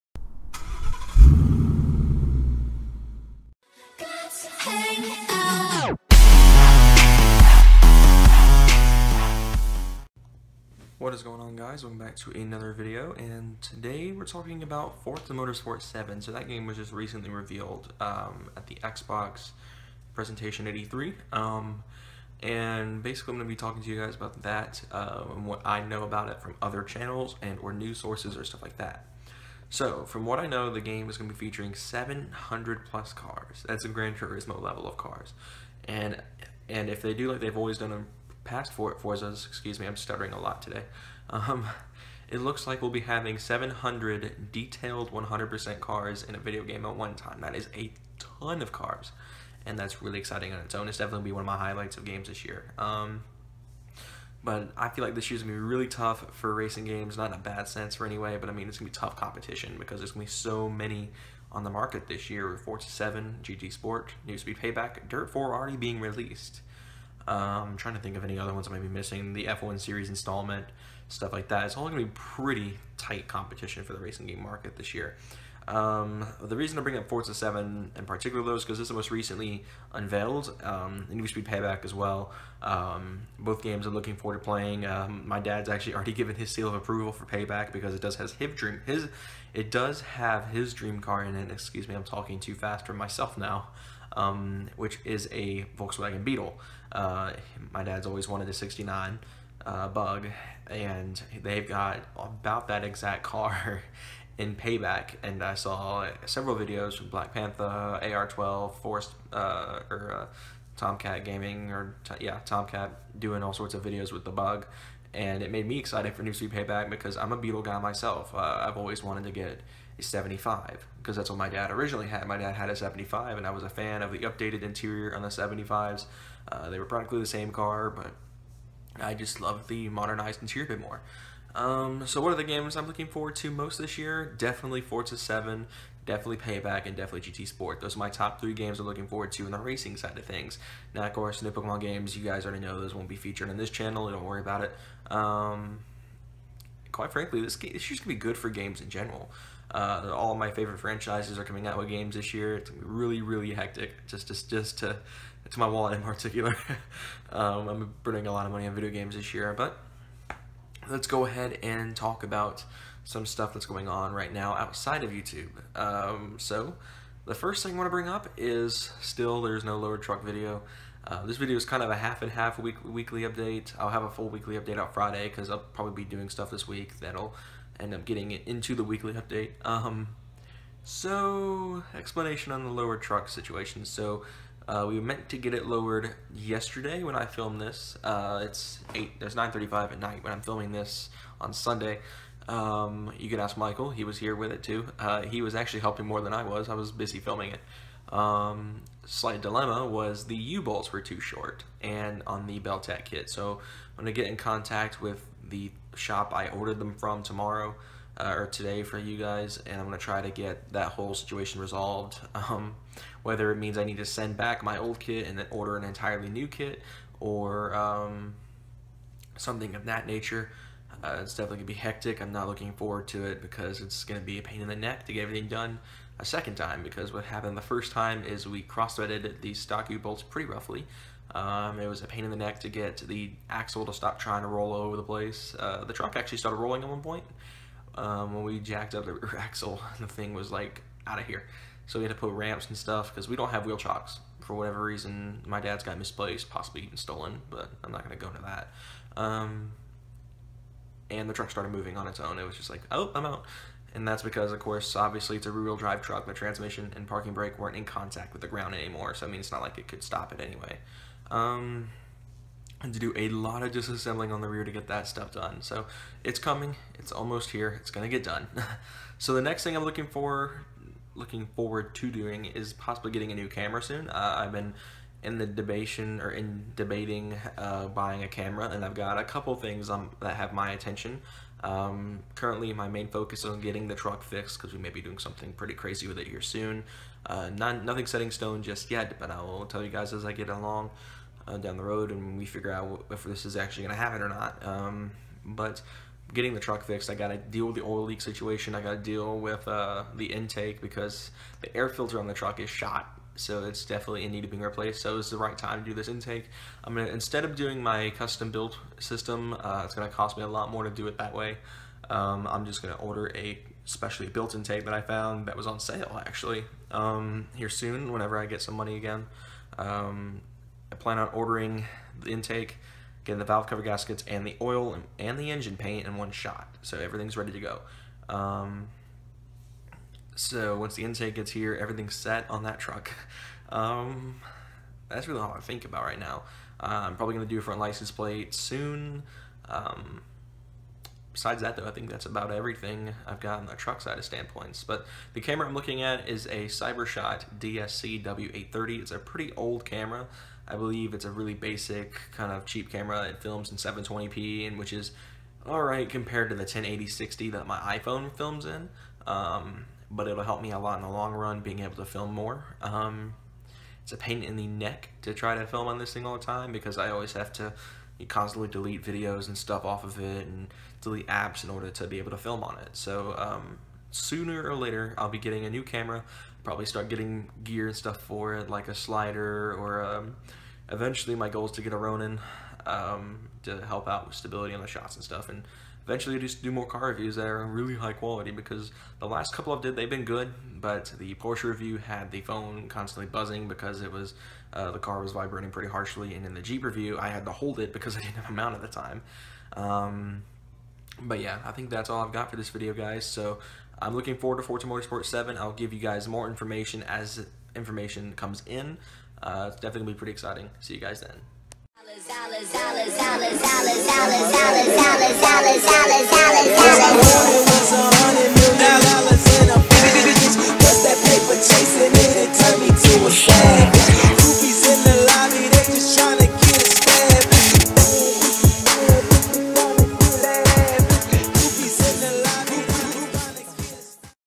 What A Year For Racing Games! Forza 7 Announced at E3!
Not exactly a trailer breakdown, may do one later though.

My PSN: jadamson1

My XBL: FordXplorer03

My Steam: RoAst_TiRez YT

Wheelwell: Jordan Adamson